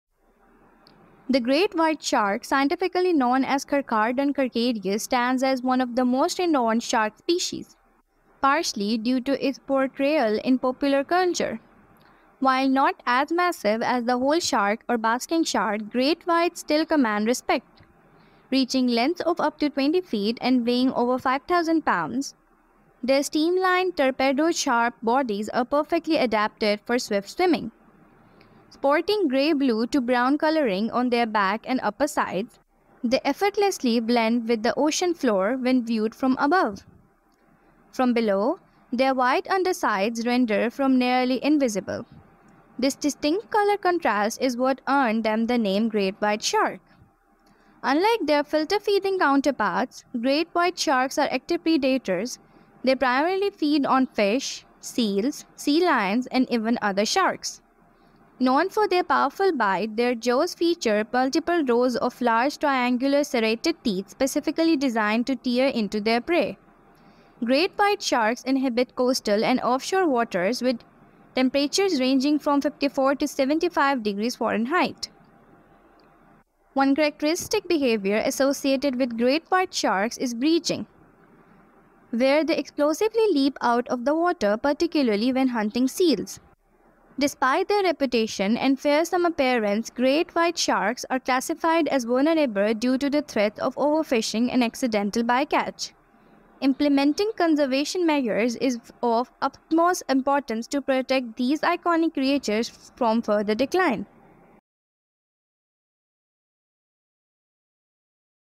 Largest Fish In The Worldgreat white shark | Rizwan Ali Tv
The great white shark, scientifically known as Carcaradon carcarius, stands as one of the most renowned shark species, partly due to its portrayal in popular culture. While not as massive as the whale shark or basking shark, great whites still command respect, reaching lengths of up to 20 feet and weighing over 5,000 pounds. Their streamlined, torpedo-shaped bodies are perfectly adapted for swift swimming. Sporting gray-blue to brown coloring on their backs and upper sides, they effortlessly blend with the ocean floor when viewed from above. From below, their white undersides render them nearly invisible. This distinct color contrast is what earned them the name "great white shark."

Unlike their filter-feeding counterparts, great white sharks are active predators. They primarily feed on fish, seals, sea lions, and even other sharks. Known for their powerful bite, their jaws feature multiple rows of large, triangular, serrated teeth specifically designed to tear into their prey. Great white sharks inhabit coastal and offshore waters with temperatures ranging from 54 to 75 degrees Fahrenheit.

One characteristic behavior associated with great white sharks is breaching, where they explosively leap out of the water, particularly when hunting seals. Despite their reputation and fearsome appearance, great white sharks are classified as vulnerable due to the threats of overfishing and accidental bycatch. Implementing conservation measures is of utmost importance to protect these iconic creatures from further decline.